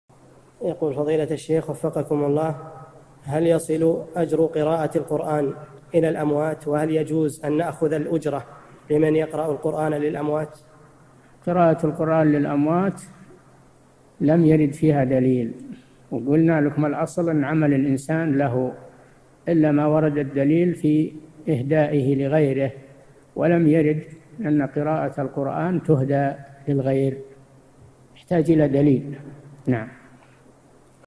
[1023 /1350] هل يصل أجر قراءة القرآن إلى الأموات؟ وهل يجوز أن نعطي الأجرة لمن يقرأ القرآن للأموات؟
-[1023 /1350] السؤال: هل يصل أجر قراءة القرآن إلى الأموات؟ وهل يجوز أن نأخذ الأجرة لمن يقرأ القرآن للأموات؟
الجواب: قراءة القرآن للأموات لم يرد فيها دليل، وقلنا لكم: الأصل أنَّ عمل الإنسان له، إِلَّا ما ورد الدليل في إهدائه لغيره، ولم يرد أنَّ قراءة القرآن تُهدى للغير، يحتاج إِلَىٰ دليل.
للاشتراك في قناة الشيخ صالح الفوزان
أسئلة وأجوبة من شرح قاعدة جليلة ( 1350 ) مقطع

للاستفسار والإقتراحات والدعم
رئيس الفريق
لمشروع كبار العلماء